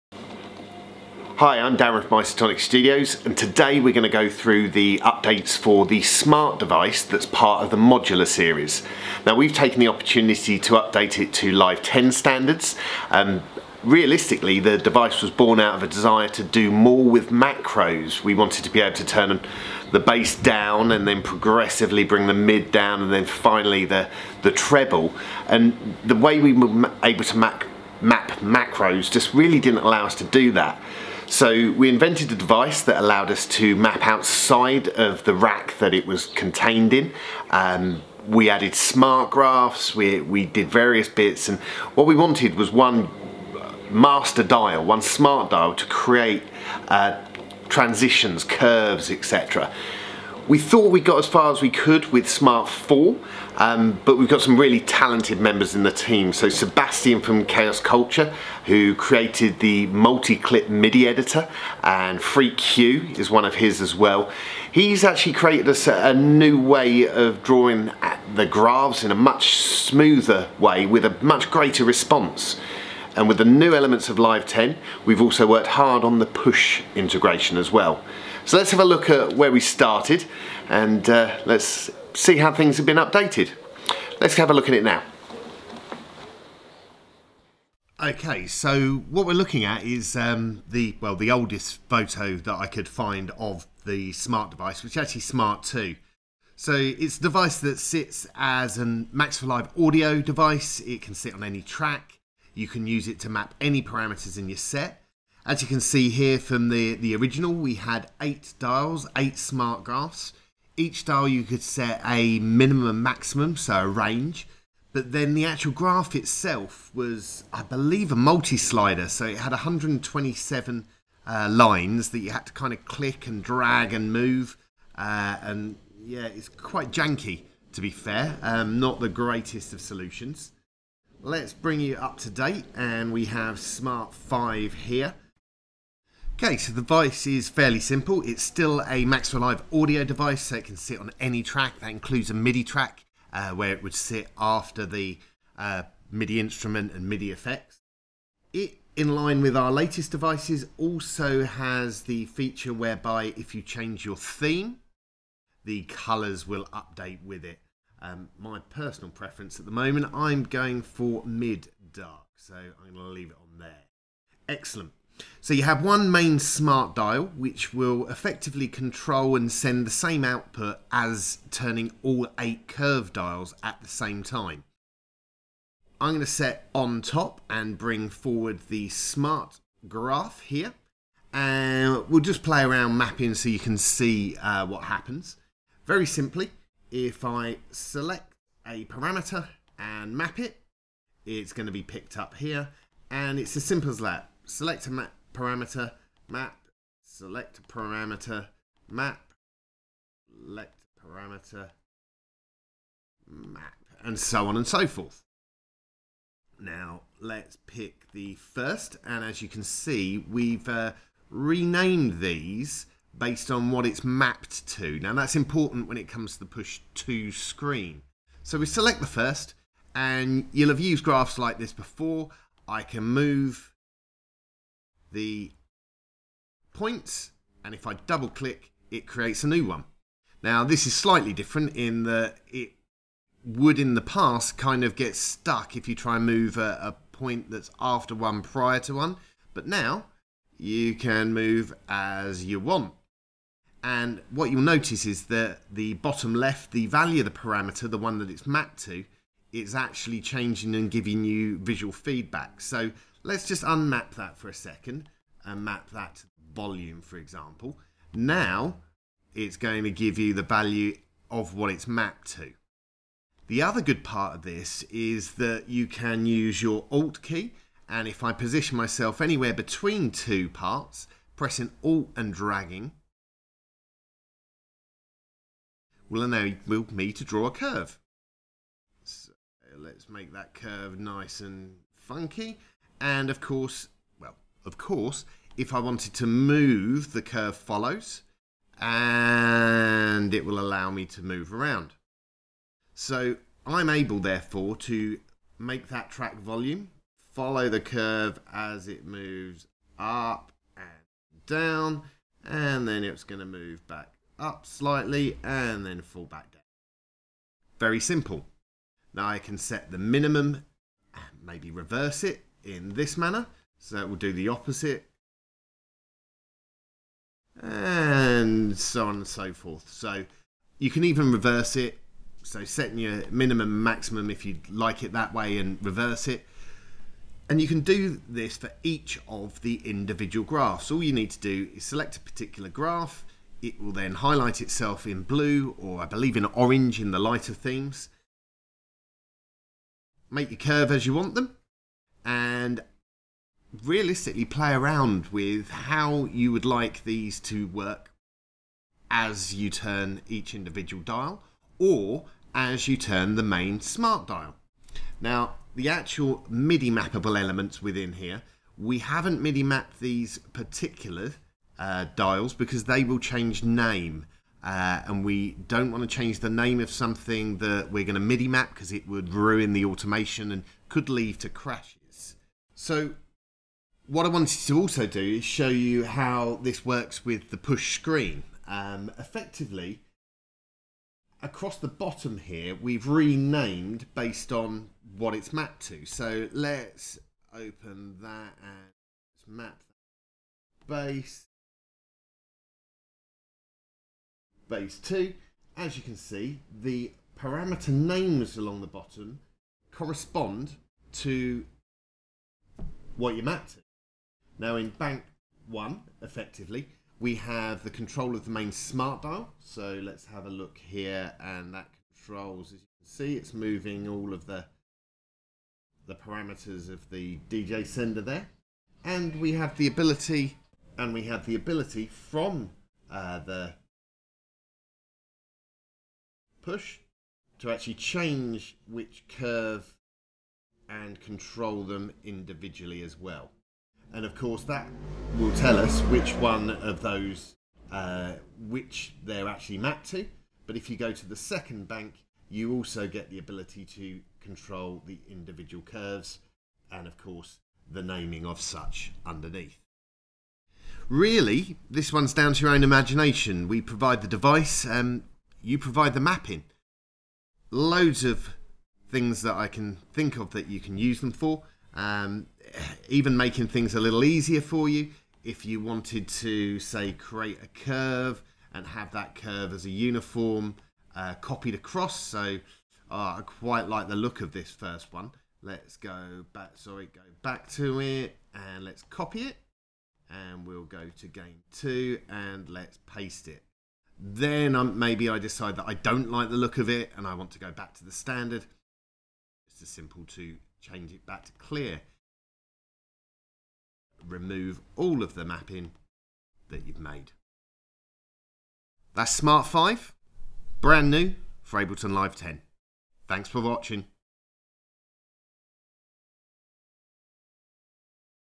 Modular Series - SMART FIVE - Advanced Parameter Control for Ableton Live 10
As part of the Modular Series, SMART has been an essential part of many Ableton Live sets over the last couple of years. We thought we'd gone as far as we could with it until now.

With this update to Live 10 standards there's tight integration with the Push including showing the mapped parameters on the screen as well as global and individual curve control.